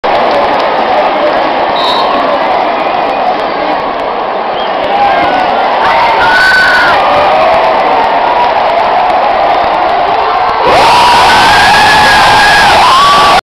Penalty Par Yoan Gouffran
Penalty Par Yoan Gouffran lors de Caen-Monaco 22/03